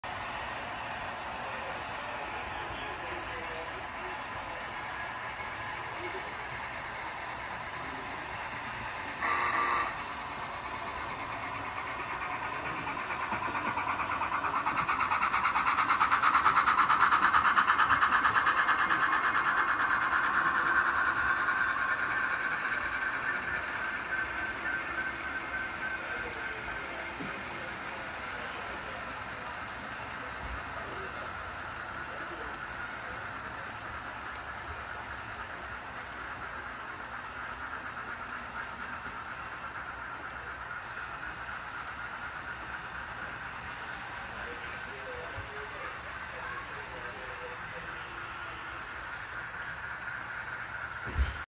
UP Big Boy Express Train With UP 1943 At Colorado Model Railroad Museum 4/28/24.
So The Next Train That We Wanted To Get Their Was UP Big Boy Steam Train With Union Pacific Sprit Of The Veteran Unit On Express Passenger Train Through The Hill.

UP Steam Express Train At 14:15pm
#4014 Steam Train